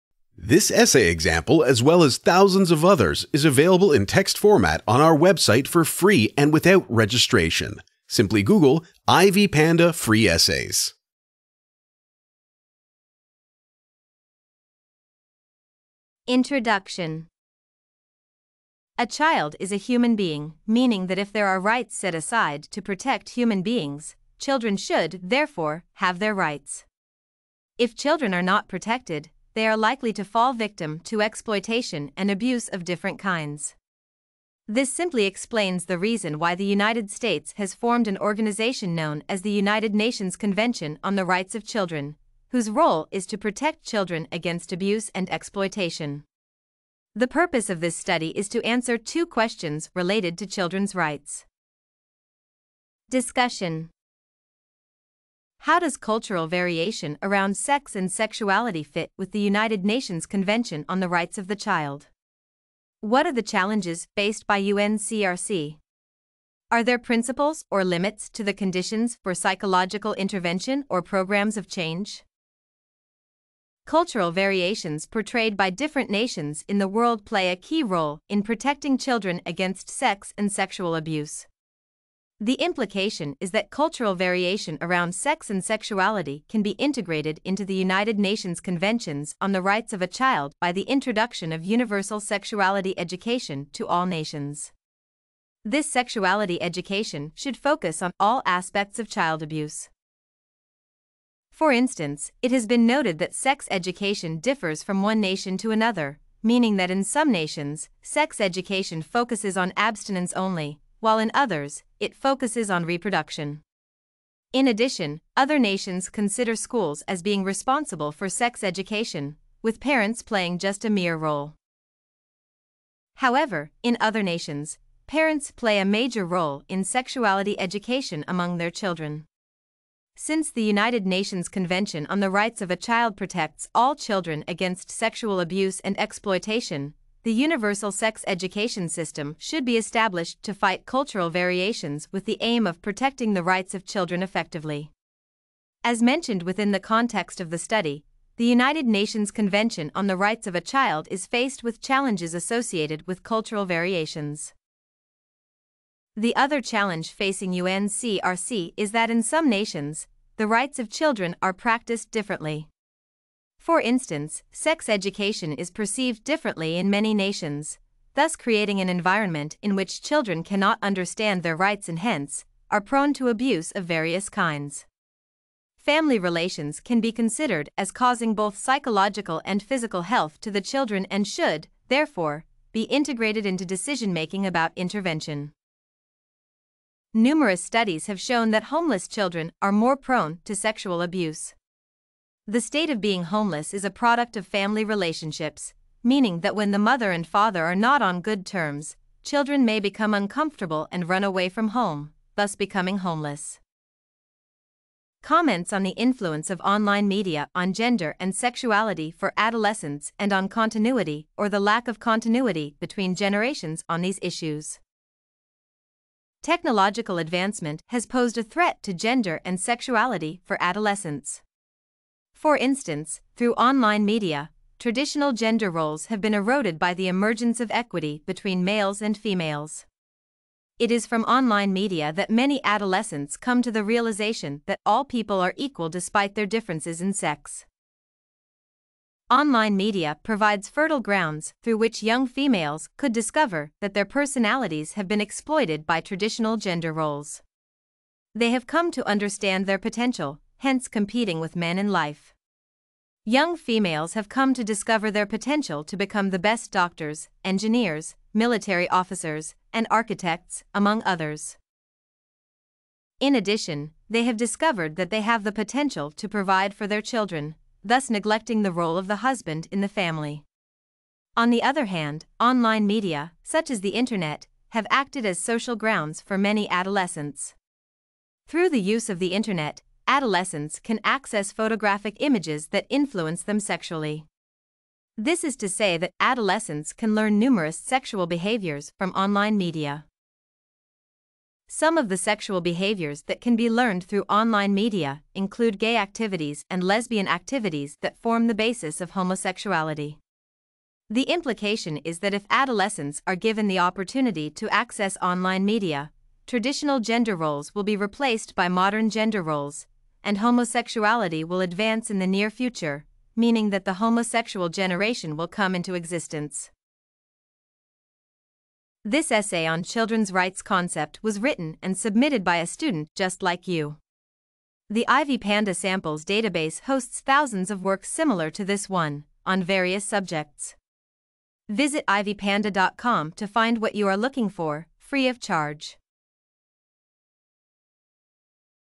Children's Rights Concept | Free Essay Example
📝 A child is a human being meaning that if there are rights set a side to protect human beings, children should therefore have their own rights.

📑 Paper type: Essay
📖 Pages: 2
🔠 Words: 726
📚 Subjects: Law, Constitutional Law
⚙️ Language: 🇺🇸 English

🔎 Works Cited:
Montgomery, Heather. An introduction to childhood: anthropological perspectives on children's lives. New York: Wiley-Blackwell, 2008. Print.